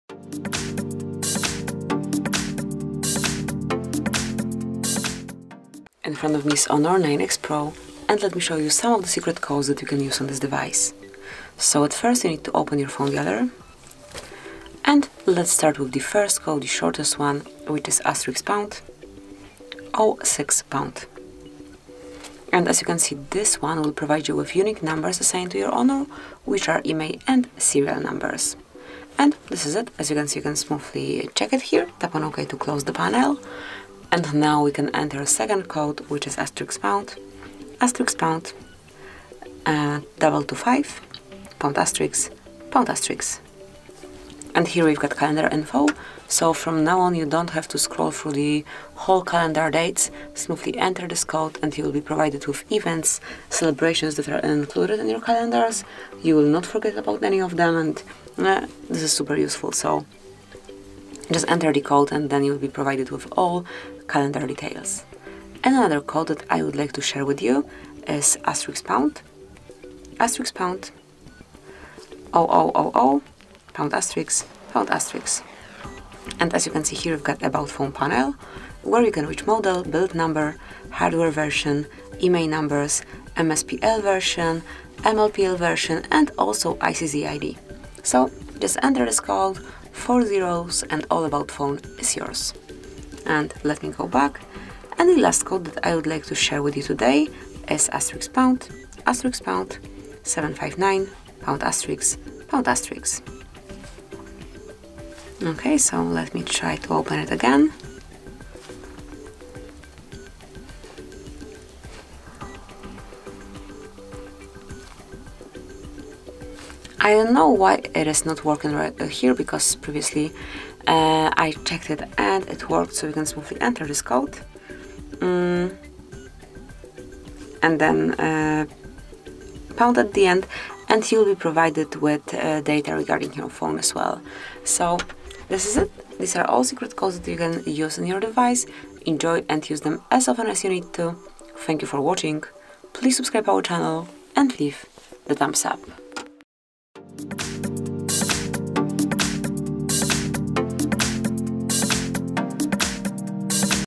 How to use Secret Codes on HUAWEI Honor 9x Pro – Open Hidden Modes and Find Hidden Options
Attention! Here you will find all the secret codes that you can enter into your HUAWEI Honor 9x Pro, so if you want to get to know your device better, stay with us. Follow the instructions and learn how to enter secret codes to reveal many hidden options and modes in HUAWEI Honor 9x Pro! Visit our YouTube channel and find a lot of useful tutorials there.